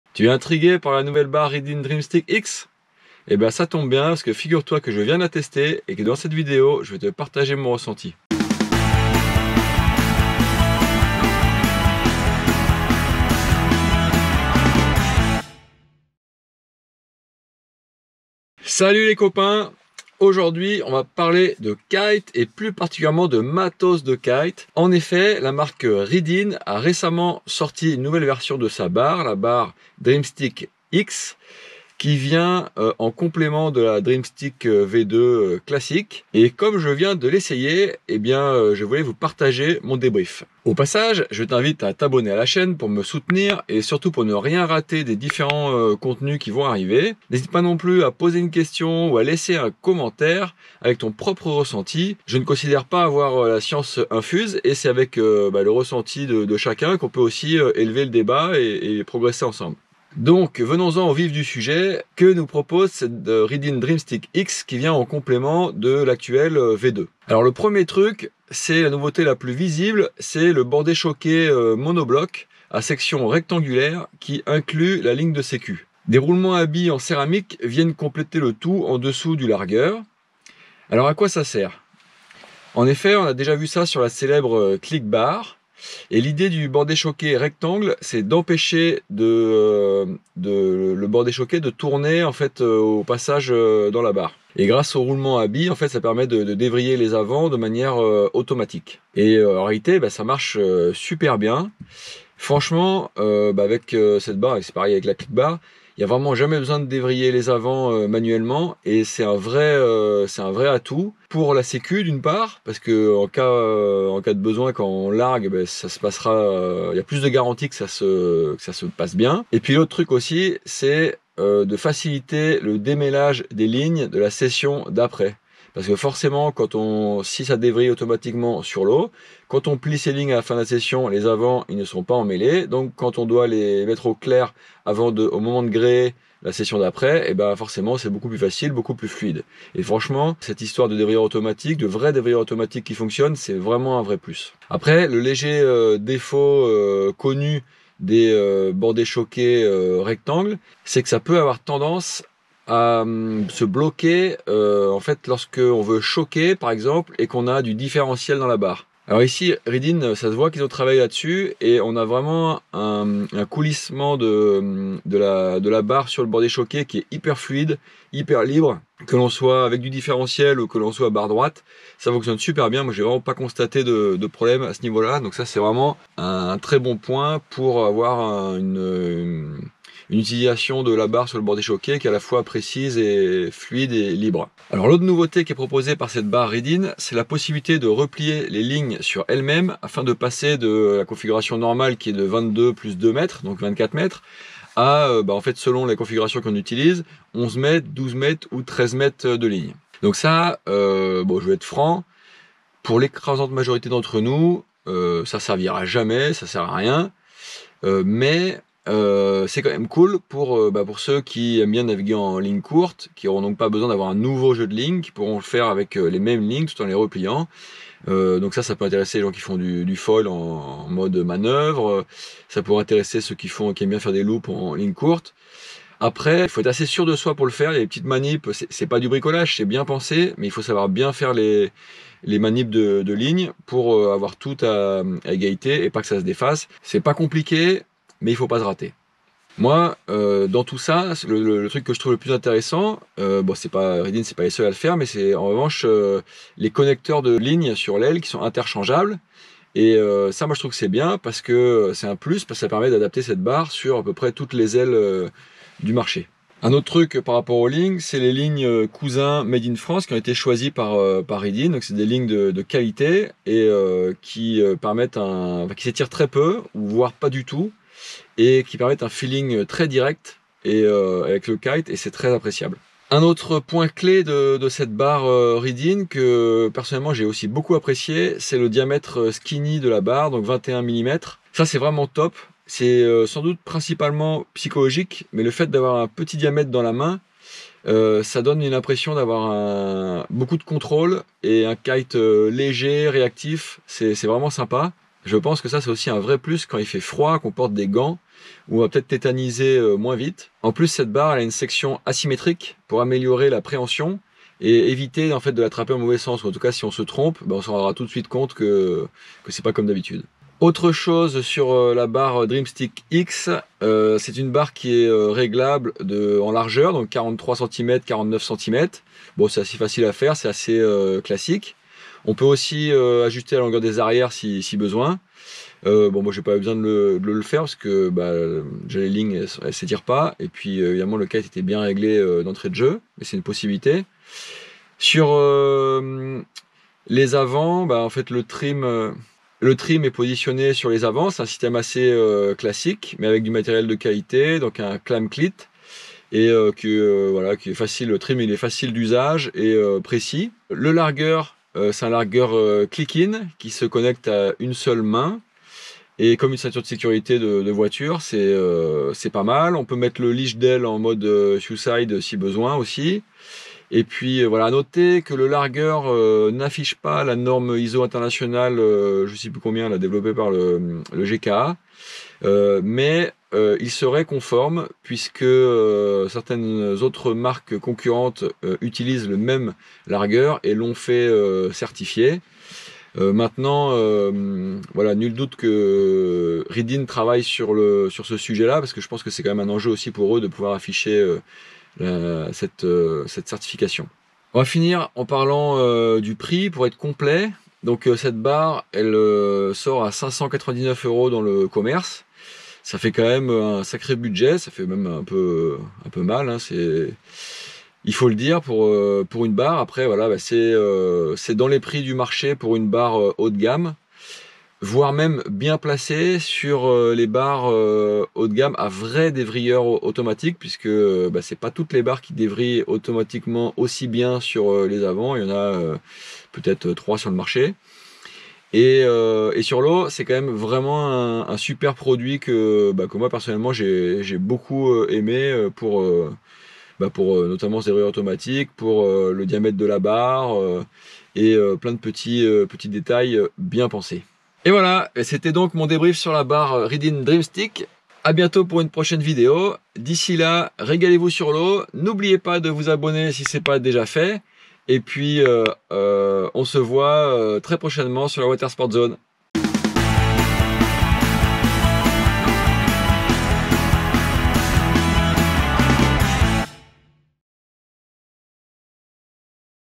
Kite test report: j'ai testé la toute nouvelle barre de Kitesurf Reedin Dreamstick X !
La marque de kite Reedin de Kevin Langeree et Damien Girardin vient de sortir une nouvelle barre de kitesurf, la Dreamstick X. Regroupant la plupart des innovations du secteur, cette barre est annoncée par la marque comme une grande avancée. Erwan Jauffroy l'a testée pour vous et il nous livre son avis en vidéo.